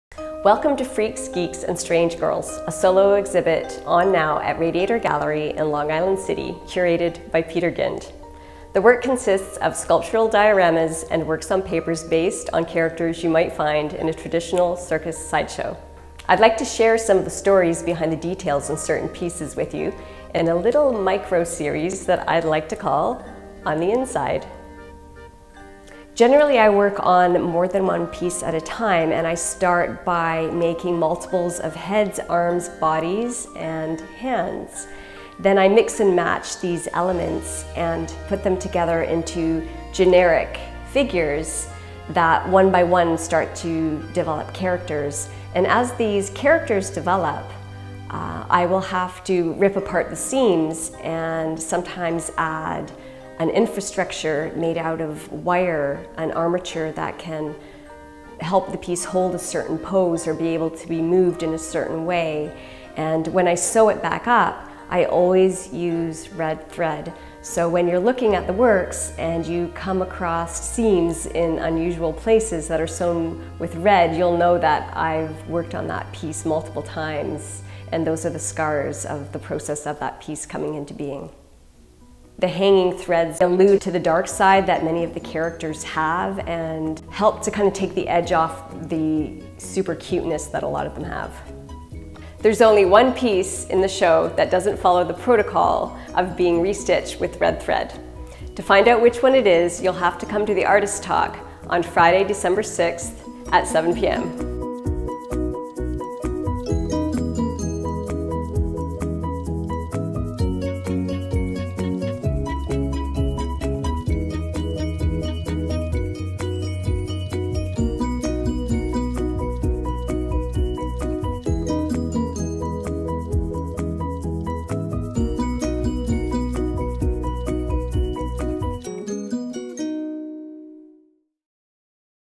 Jody MacDonald ~ On the Inside ~ The Red Thread
Please join
curator, Peter Gynd and I
Friday, December 6th, 7pm
at Radiator Gallery
we'll be discussing the process and details behind
the densely layered works on view in Freaks, Geeks, and
Strange Girls.

Radiator Gallery,
10-61 Jackson Ave.
3rd floor
Long Island City, NY.

I couldn't be happier with how the work has been received and
I'm thrilled to share this lovely mention in Hyperallergic
by Alissa Guzman.

Gallery Hours:
Fridays and Sundays, 1-6pm, and by appointment
On view November 15th, 2019 through January 17, 2020.

Getting There:
The gallery is readily accessible by transit.
The Vernon Blvd - Jackson Ave stop on the 7 train line
will get you within 2 minutes walking distance.

Alternately, the gallery is a 10 minute walk along
Jackson Ave from Court Square Station
(M, G, E, and 7 trains).